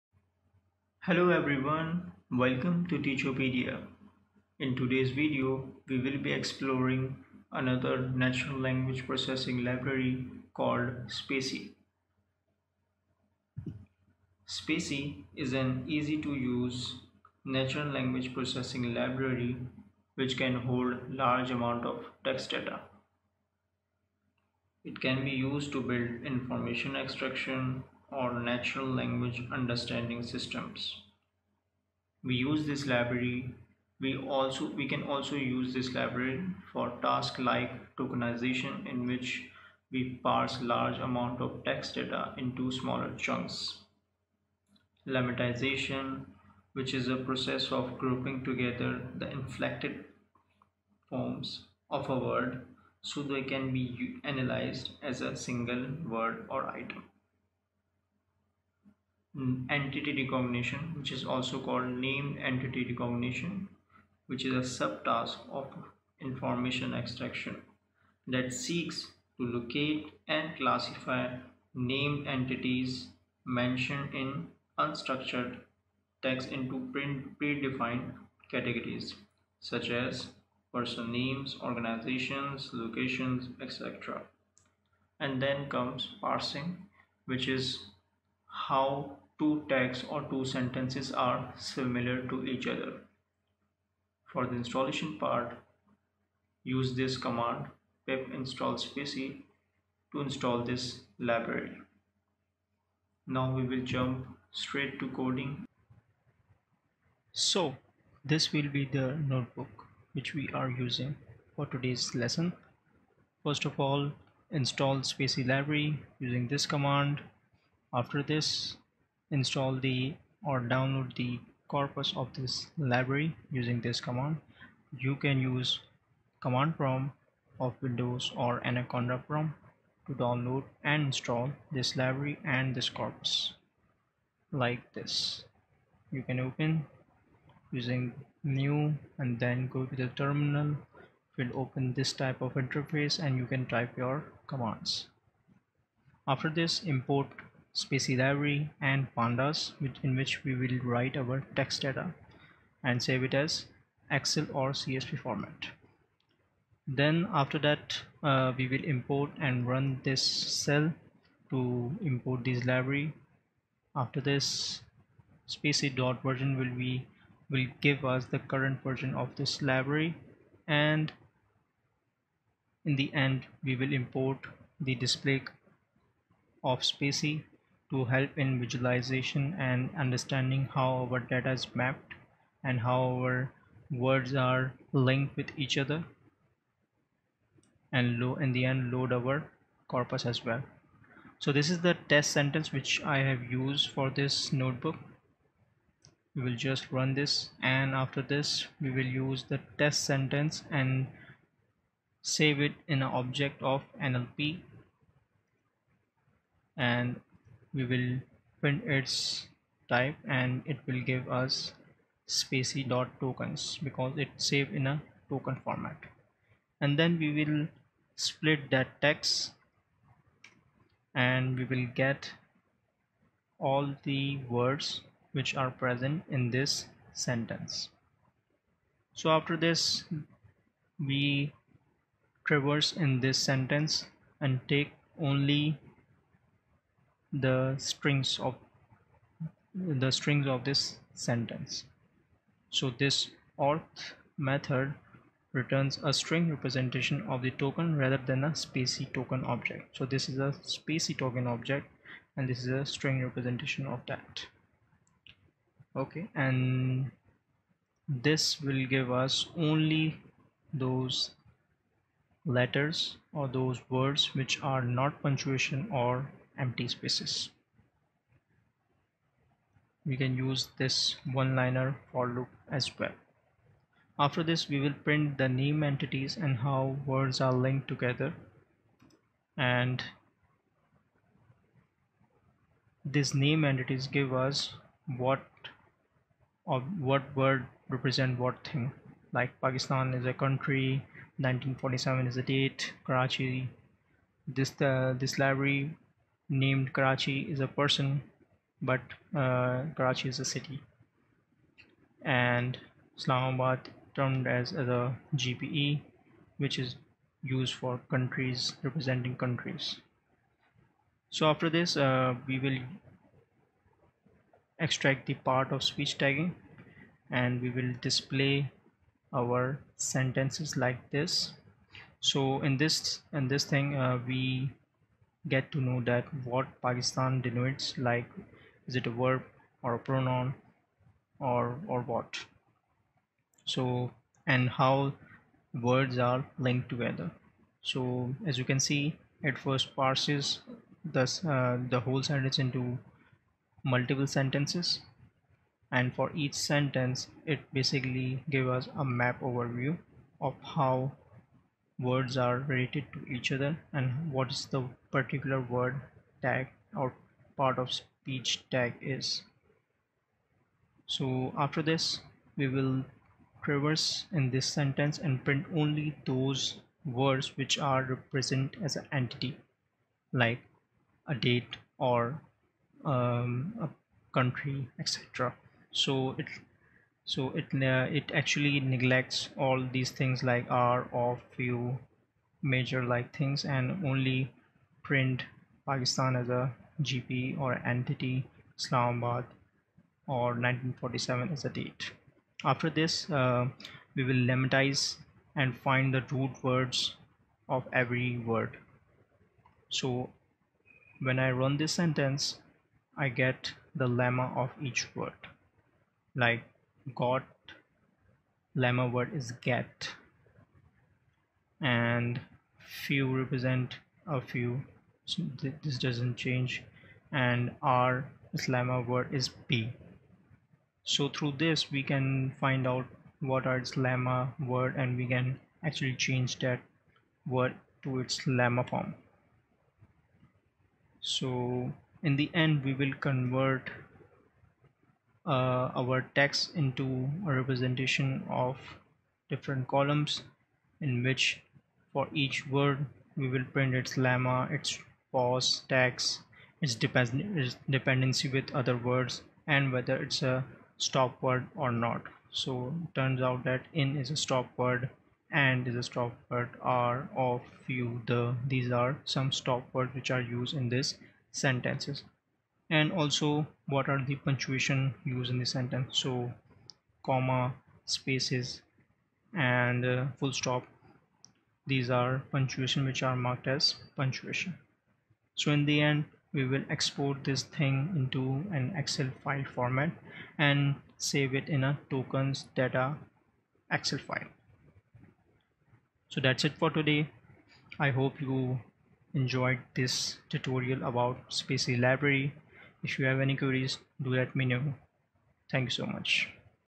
Natural Language processing with Python | Introduction to Spacy
This video will give an overview about a Natural Language processing library called Spacy.
Its one of the popular and easy-to-use natural language processing library in Python. It helps in building applications that can process and get insights from large volumes of text. It can be used in task related to information extraction or natural language understanding systems, deep learning etc.